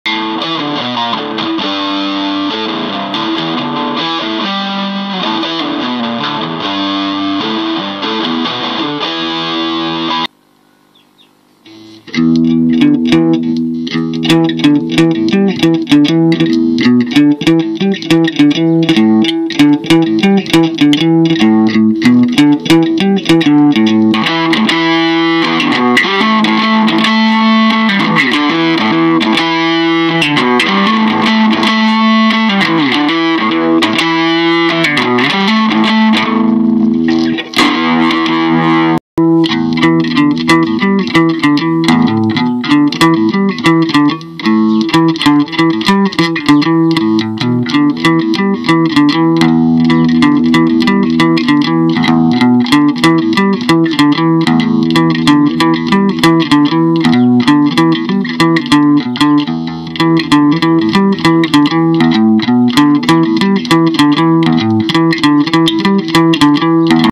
The last part improvising only on two remaining guitar strings
My last video, I spontaneously tried to improvise on only two strings, A + E more was not left in the moment on my electric guitar ...